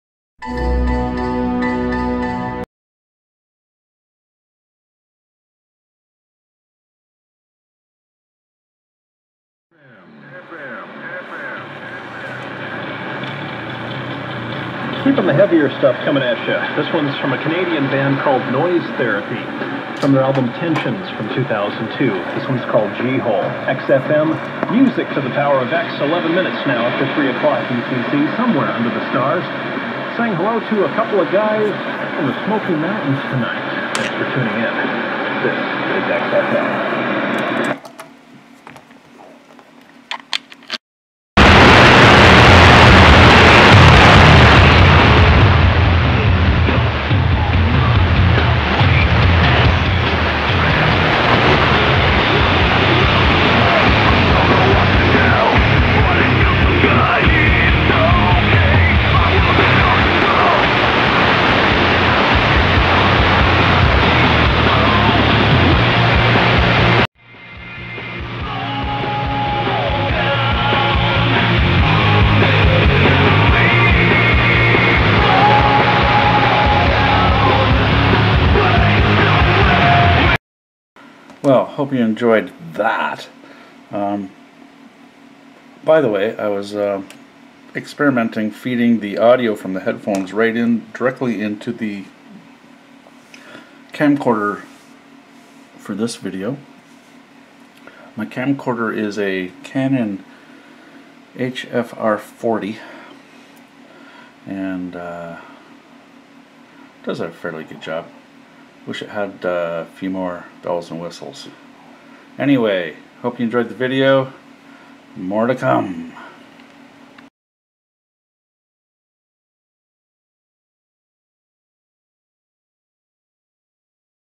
XFM pirate radio
Here is XFM (transmitting in AM), a pirate from somewhere under the stars.

Testing feeding the audio directly into my Canon HF-R40 camcorder.